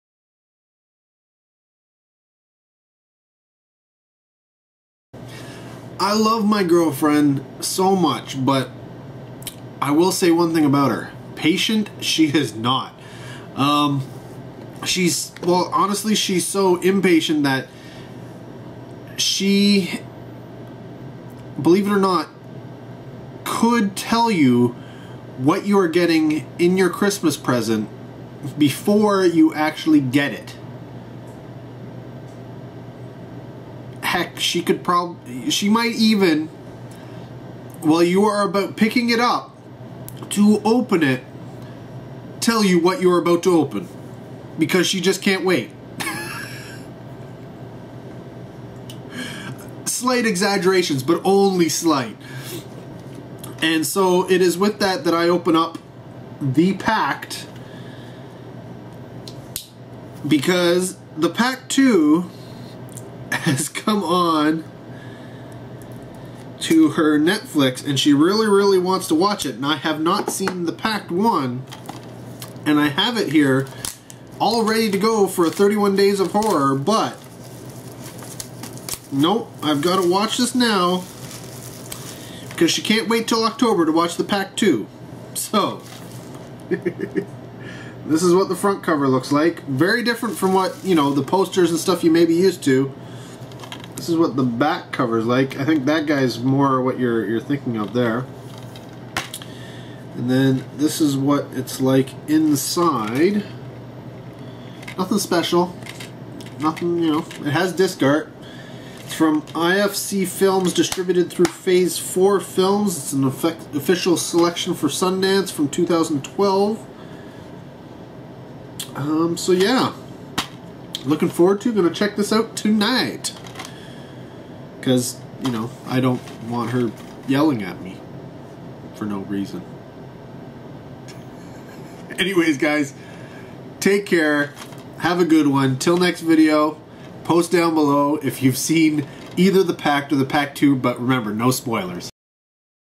RobVlog - Unboxing the DVD of The Pact
Though I was saving this one for a 31 Days of Horror one year... Lesley decided I had to watch it now... like right now.
So I decided why not unbox the release and show everyone at the same time?
So here's my unboxing of the DVD of The Pact from Phase 4 Films